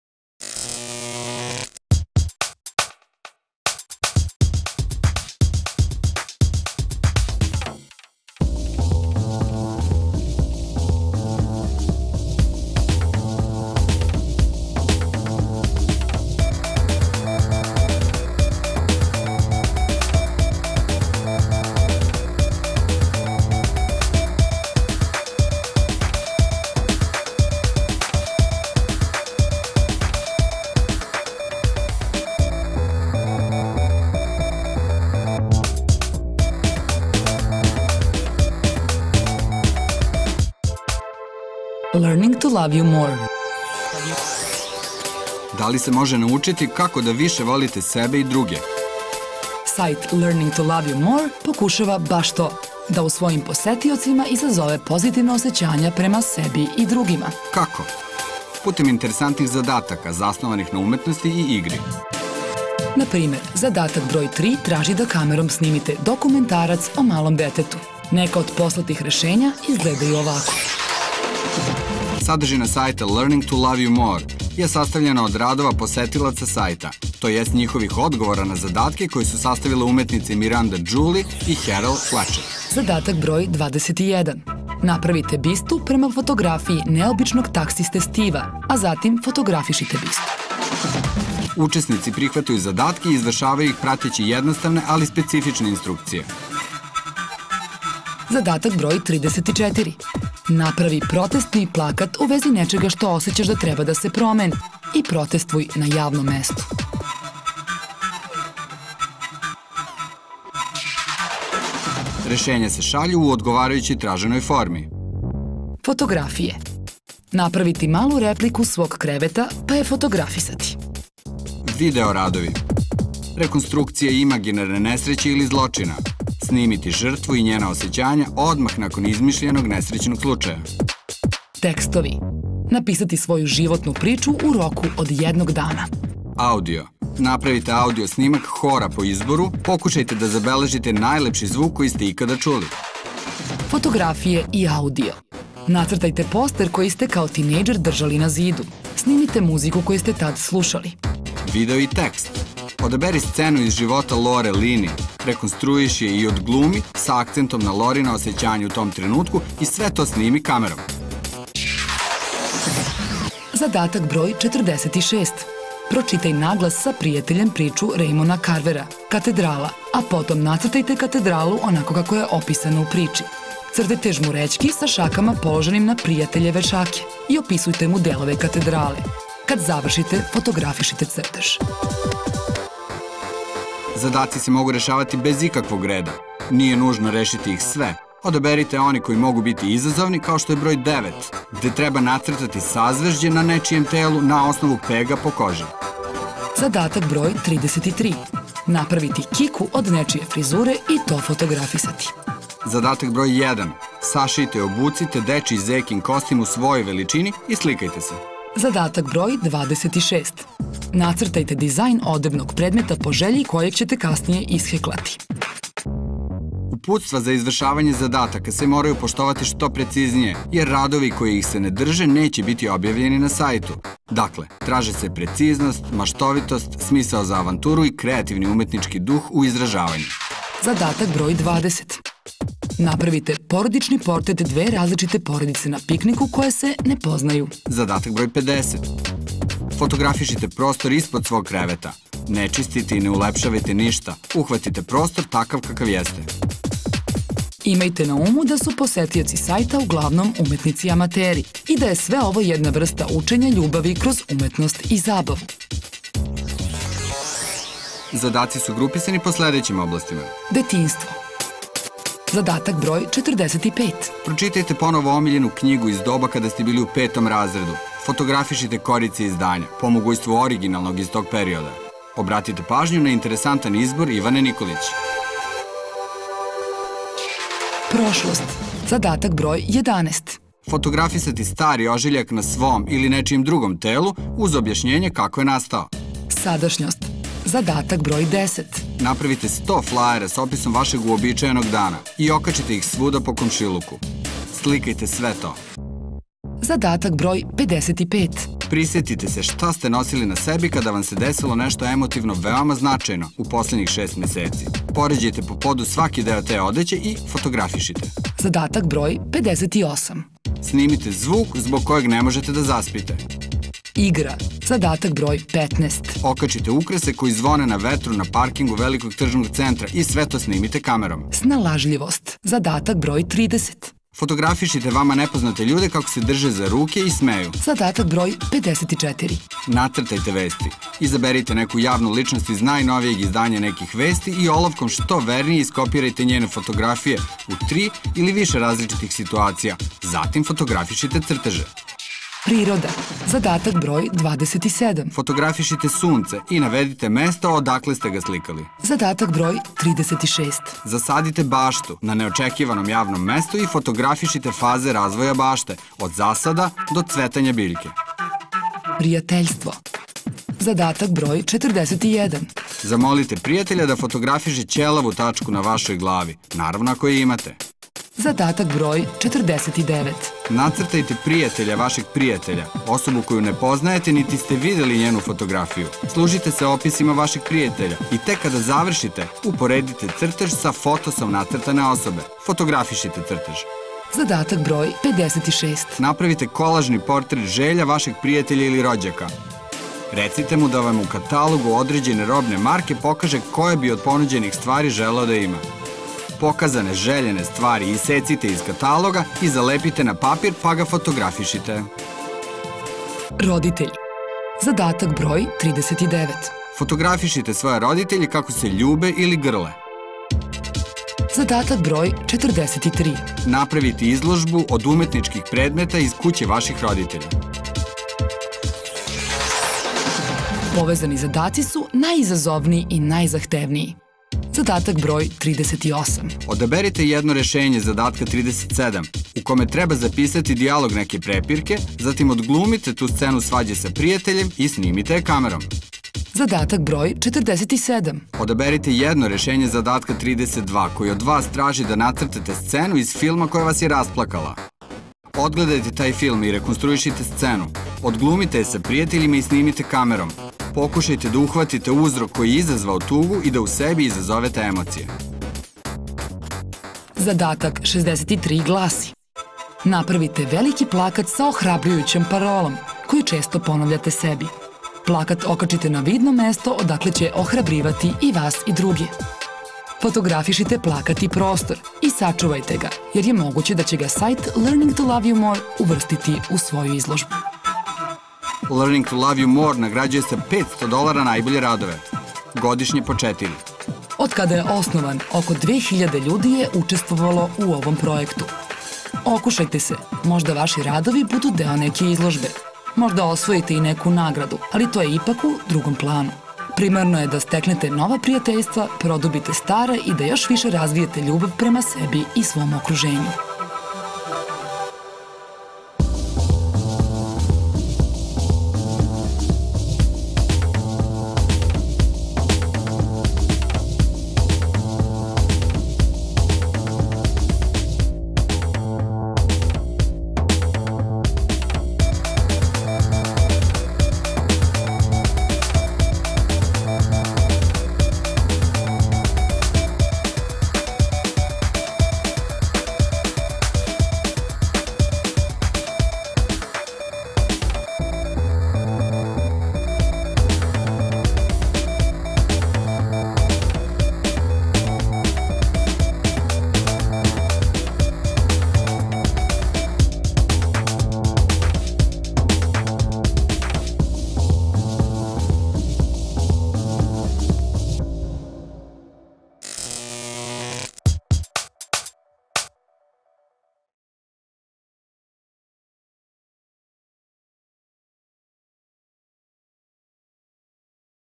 tvwww 33 learningtoloveyoumore
Da li se može naučiti kako da više volite sebe i druge?
Sajt LEARNING TO LOVE YOU MORE – pokušava baš to – da u svojim posetiocima izazove pozitivna osećanja prema sebi i drugima.
Kako?
Putem interesantnih zadataka, zasnovanih na umetnosti i igri.
Okušajte se, možda vaši radovi budu deo neke izložbe, možda osvojite i neku nagradu, ali to je ipak u drugom planu.
Bitno je da steknete nova prijateljstva, produbite stara i da još više razvijete ljubav prema sebi i svom okruženju.

Is it possible to learn how to love more yourselves and others?
Website LEARNING TO LOVE YOU MORE - tries just that - to induct positive feelings to their visitors toward themselves and others through interesting tasks, based on the art and games.